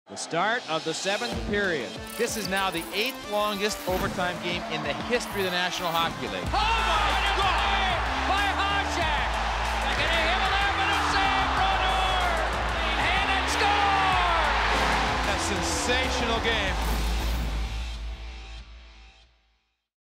Hasek makes 70 saves in four overtimes | This Date in History
On April 27, 1994, Dominik Hasek outduels Martin Brodeur, making 70 saves in the 4-overtime 1-0 win.

Feeling social?